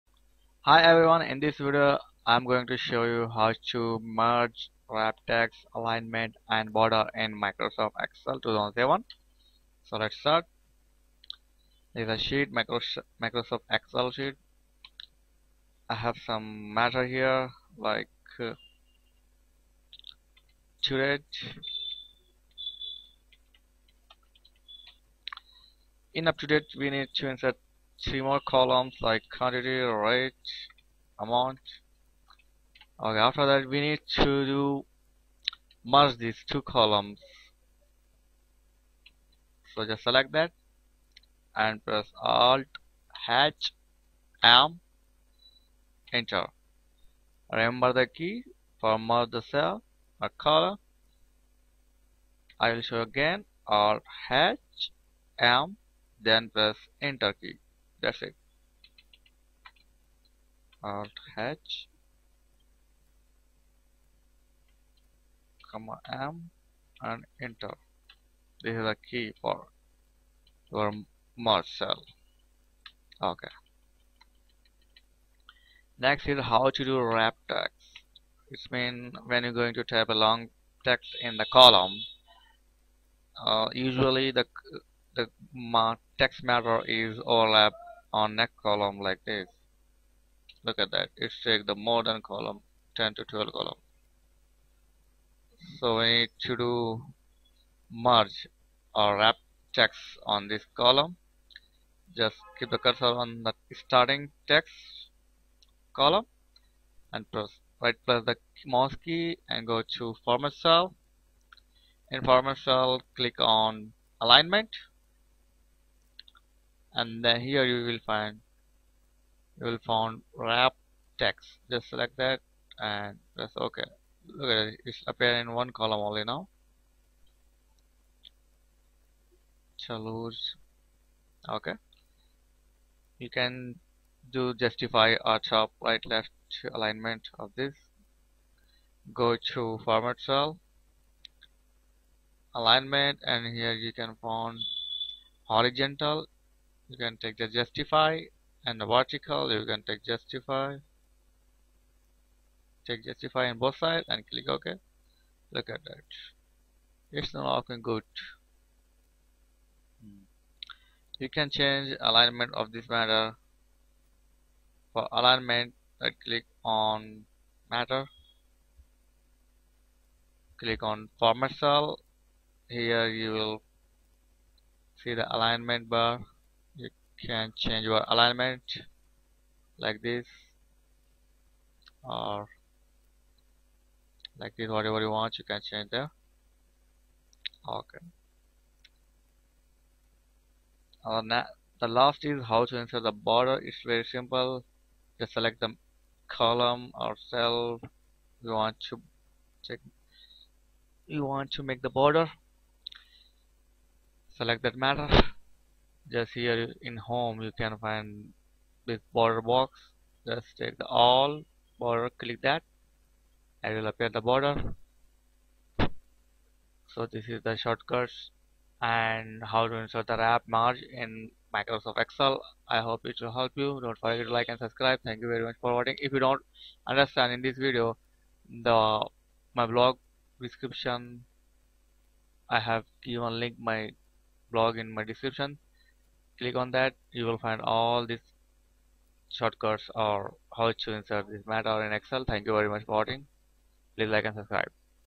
Microsoft excel shortcut: How to do merge, wrap text, alignment & border (5of5)
In this video you will learn how to merge cell, wrap text cell, cell alignment and how to insert border in Microsoft excel